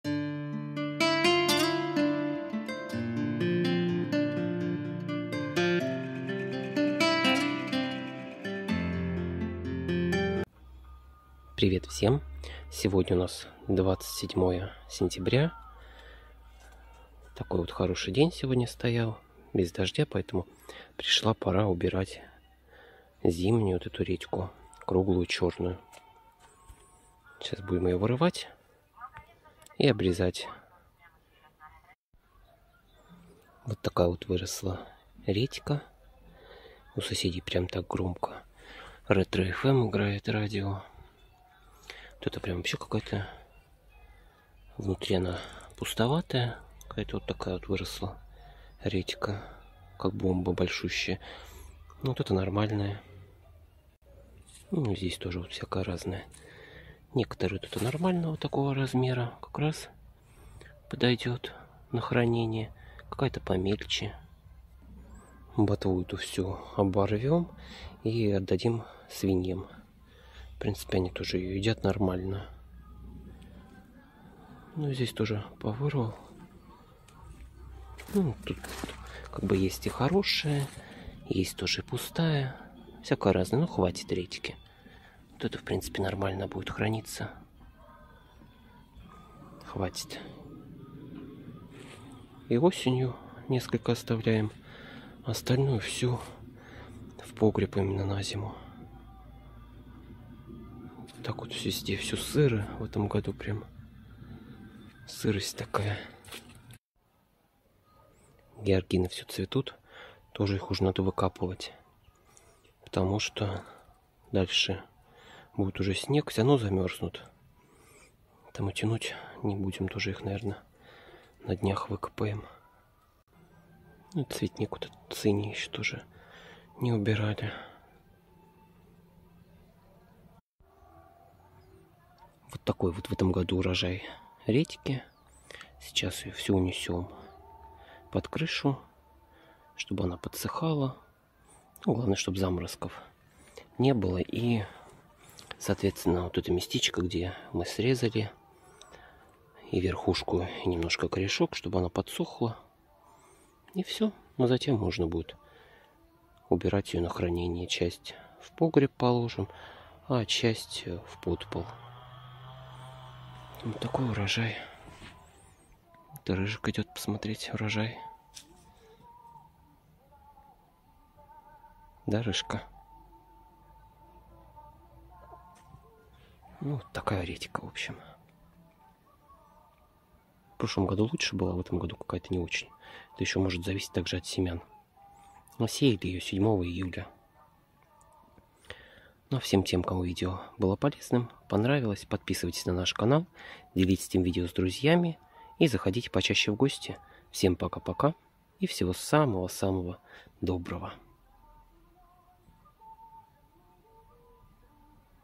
Убираю редьку круглую зимнюю черную
Урожай готов, пора убирать! Очень полезная редька!